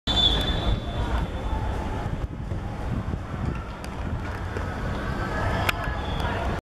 大埔vs葵青(2015.3.6.青少年足球U18乙組聯賽)之入球3:0
大埔7號